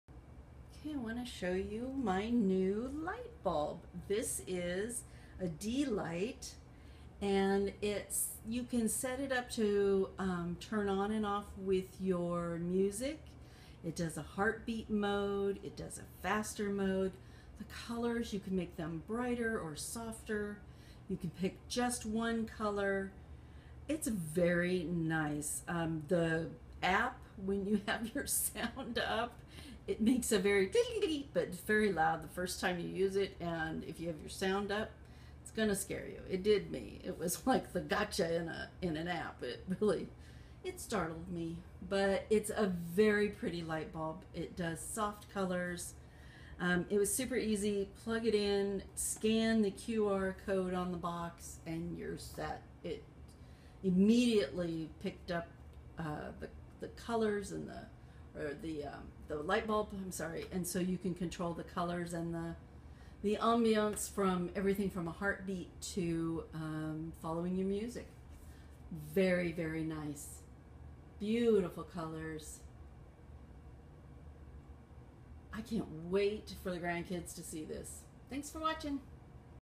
ICherir Miracle Music LED light bulb
The follows the beat of the music or whatever mood you are in. There is even a heartbeat mode. The bulb puts out beautiful colors and you control the brightness with the app on your smart device.